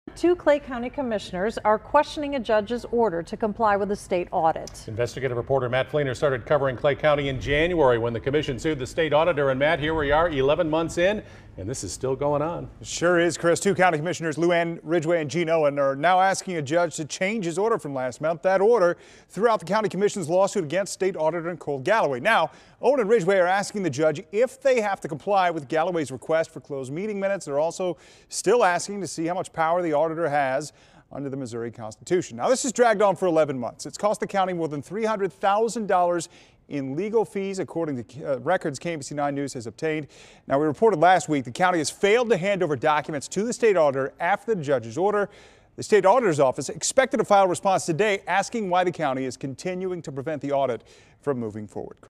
2 Clay County Commissioners question judge's order on citizen-mandated audit
Clay County Commissioners Luann Ridgeway and Gene Owen are questioning a judge's order concerning a citizen-mandated audit.